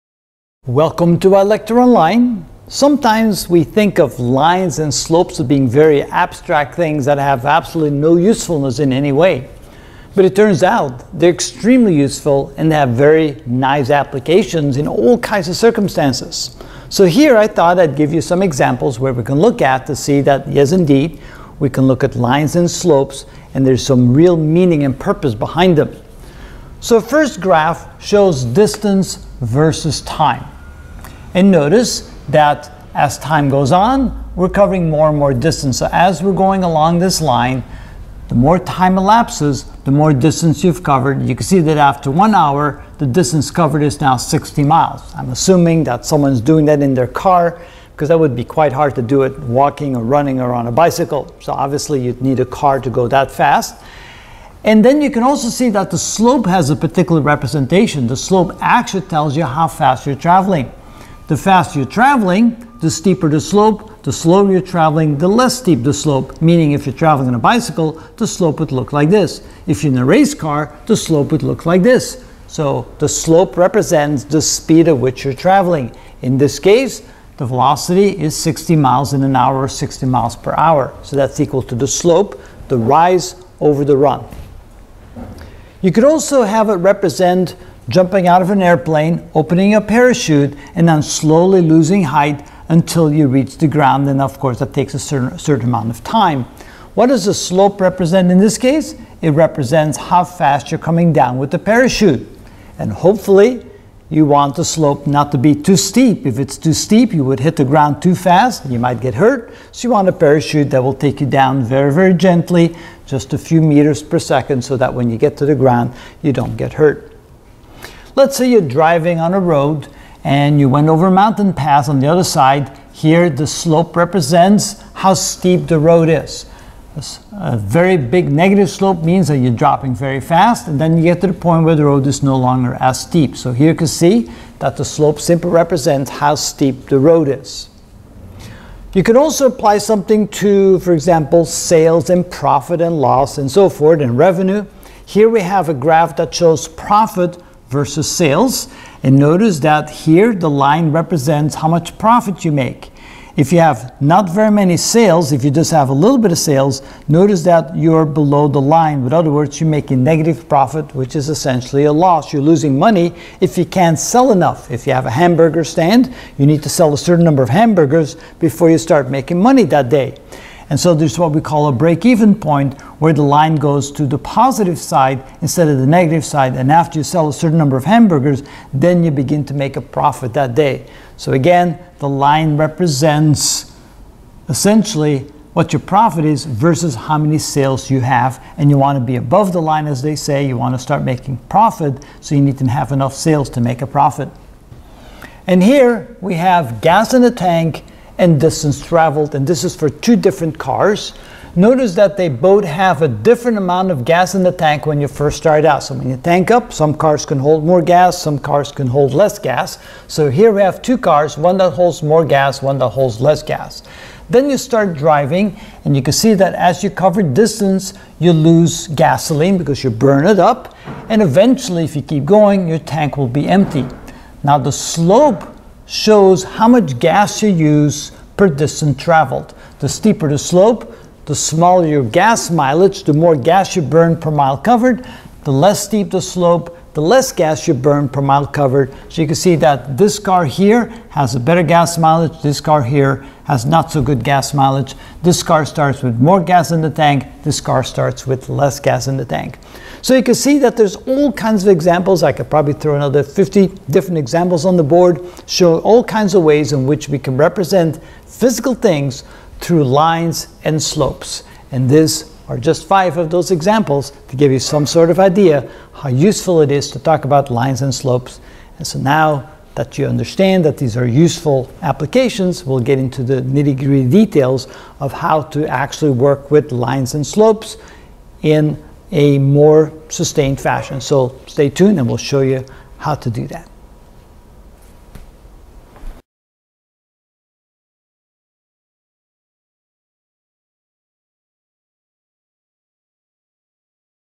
Geometry - Ch. 4: Lines and Angles (47 of 54) How to Interpret Lines and Slopes' Meaning
We will learn how to interpret and uses for graph of lines and slopes: gas in tank vs distance traveled, elevation vs distance slope=steep road, distance vs time slope=velocity, profit vs sales slope=break-even point, and height vs time slope=open the parachute.

Title:  Geometry - Ch. 4: Lines and Angles (46 of 54) Find the Slope Perpendicular to the Line

Title:  Geometry - Ch. 4: Lines and Angles (48 of 54) What is an Equation of Line?